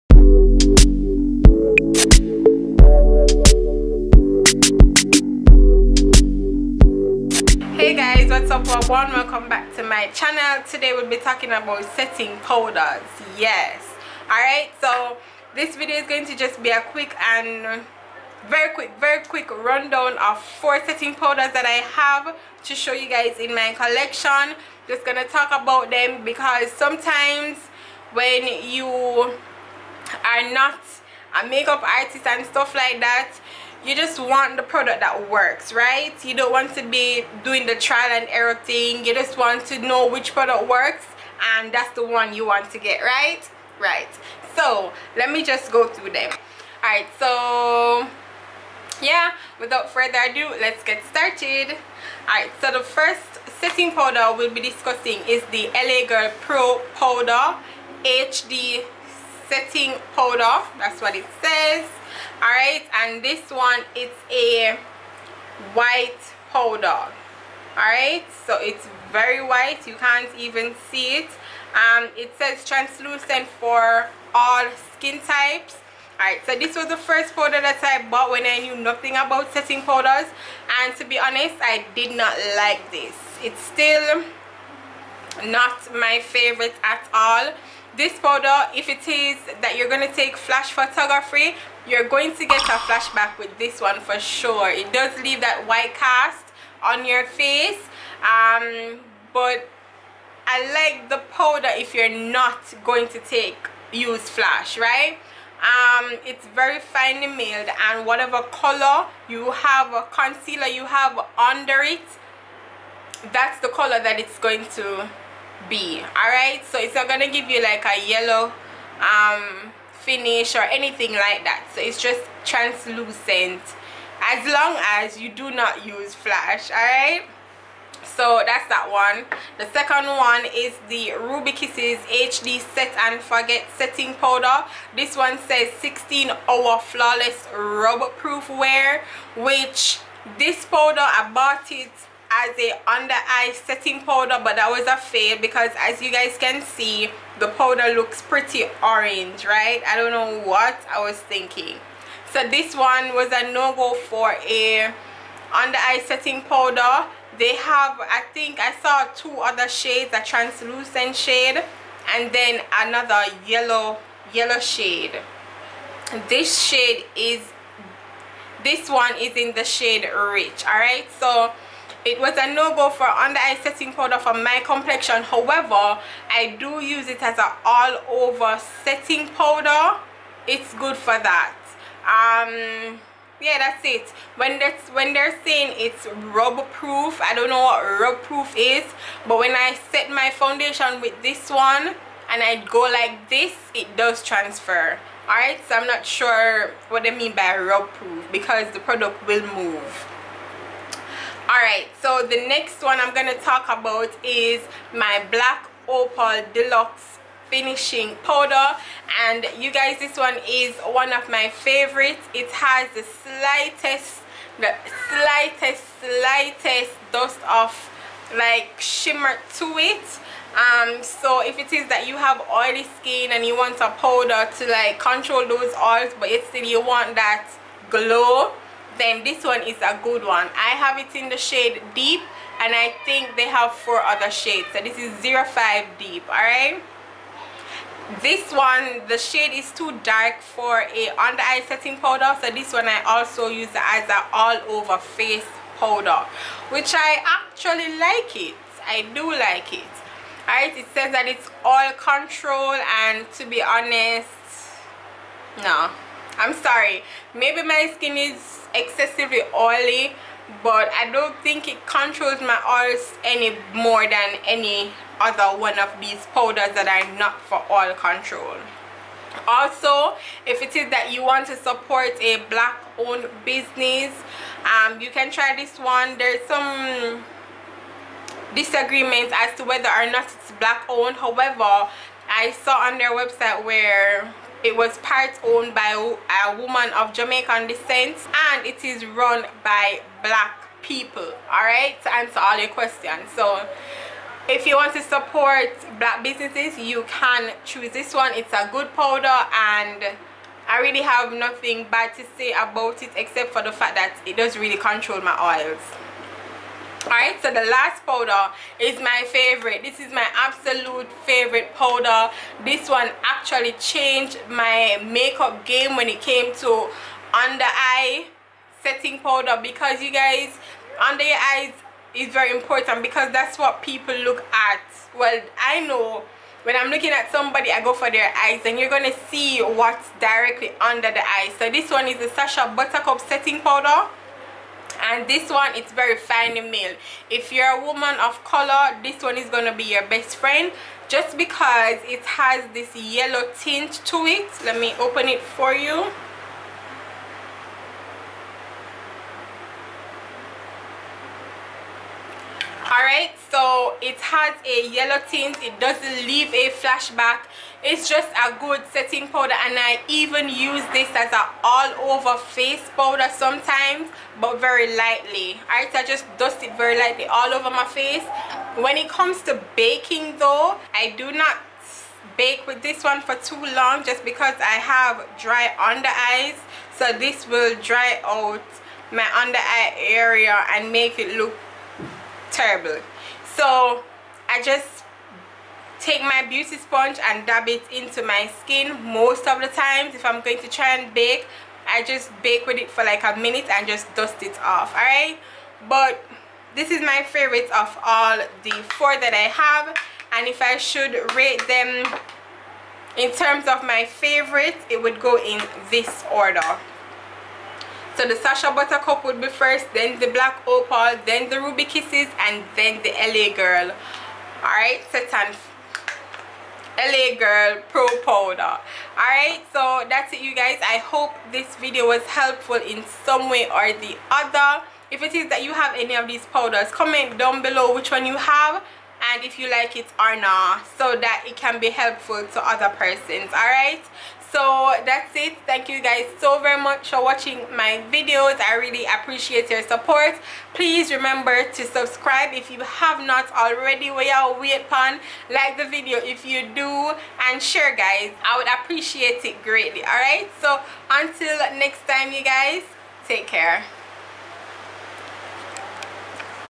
💕 Setting Powder Review || Which IsThe Best Setting Powder?
Hey Loves ❤️
This is my review of 4 setting powders; The Sacha Buttercup Setting Powder, Black Opal Deluxe Finishing Powder, LA Girl Pro HD Setting Powder  and Ruby Kisses Set and Forget Setting Powder.

Ruby Kisses Set and Forget Setting Powder - USD 8.27

OR